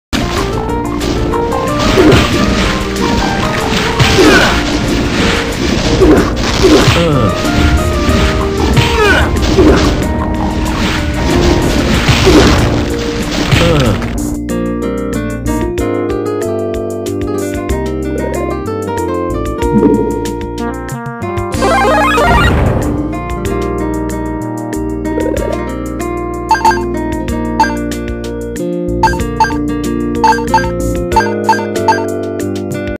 All I want for Christmass is my big Brass Knuckles
lol Beat the piss outta a few Imps with the Santa hats, tiz the seazon for gore!